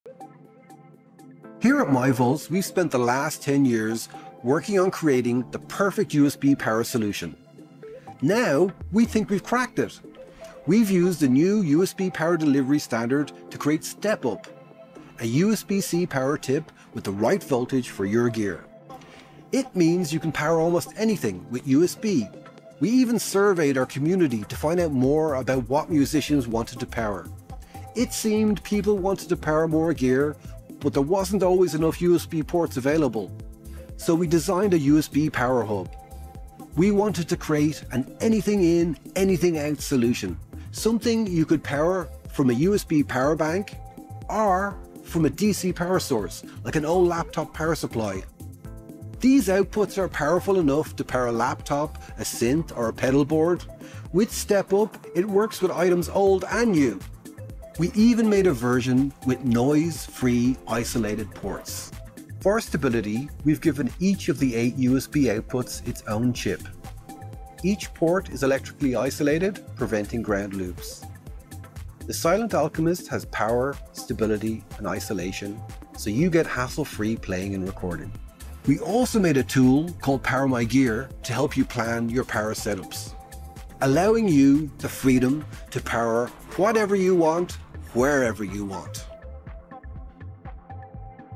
The Alchemist - USB Power all your gear with this high power isolated hub
Live now on Kickstarter:
The ultimate USB Power Hub. Ideal for musicians and mobile Creatives.
Power a string of synths, pedals, your phone, laptop.
All your Digital and Analog devices powered simply and efficiently.